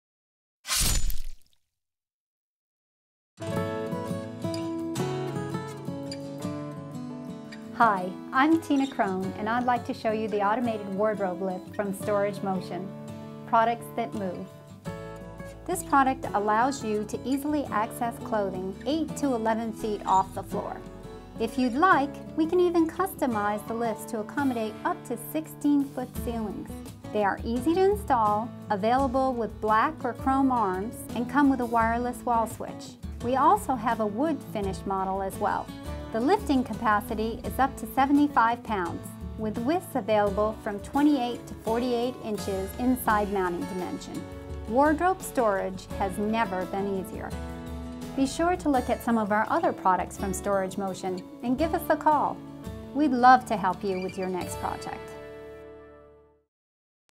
Automated Wardrobe Lift ; An electric motor driven clothes rail or closet rod
This  Automated Wardrobe Lift is motor driven and lowers and raises hanging clothes over 50 inches. It comes with a  wireless wall switch and is powered by a well hidden drive system. This product is from StorageMotion Inc